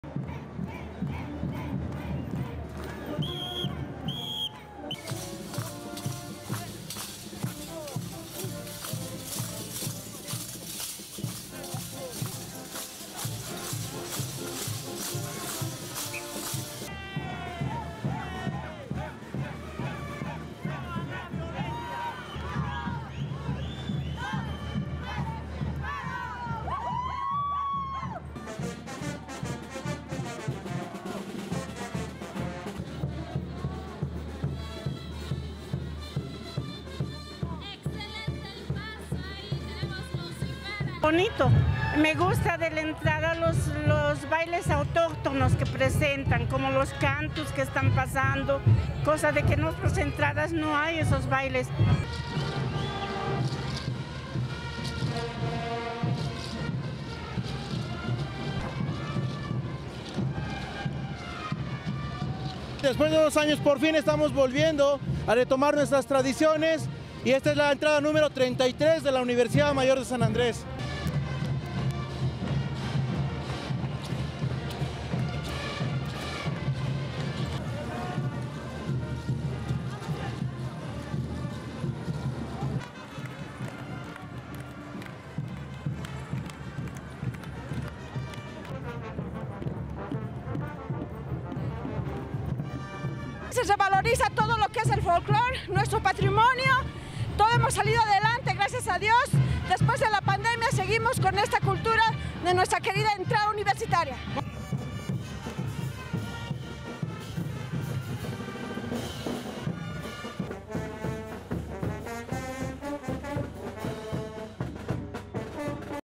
Students flock to La Paz streets to dance folk
ASSOCIATED PRESS
La Paz - 22 October 2022
1. Various of dancers from a students' fraternity performing throughout a central avenue during the annual University Folkloric Entrance
2. Audience cheering
3. Women dancing, wearing Bolivian folk dresses and holding sticks with horns
4. Bolivian folk characters of black and white bears dancing
5. Members of a fraternity dancing
6. SOUNDBITE (Spanish) Rosa María (surname not given), Bolivian folk dances enthusiast:
"I like the native dances they're representing. And also the songs, you don't find them elsewhere."
7. Bolivian folk character of Old Man dancing
8. Student Juan José Lara dancing
9. SOUNDBITE (Spanish) Juan José Lara, law student and folk dancer:
"After two years, we are finally coming back, returning to our traditions. This is the 33rd entry of the Higher University of San Andres."
10. Various of students dancing during the folk fair
11. SOUNDBITE (Spanish) Lisbet Quisbet, student and dancer:
"Here everything that is folklore, our heritage, is revalued. Thank God we have come through the pandemic (of COVID-19). We continue with our culture and our beloved University Folkloric Entrance."
12. Various of students dancing
STORYLINE:
Thousands of dancers turned out to the streets of La Paz on Saturday to celebrate the annual University Folkloric Entrance, the traditional students' folk fair.
Sixty-two dancing groups representing fraternities participated in the event, organized by the Bolivian Higher University of San Andres.
The parade, which highlights Bolivian tradition and heritage, was cancelled for three years during the COVID-19 pandemic.
The first University Folkloric Entrance was held in 1988 and comprised just eight different folkloric groups.
AP Video by Carlos Guerrero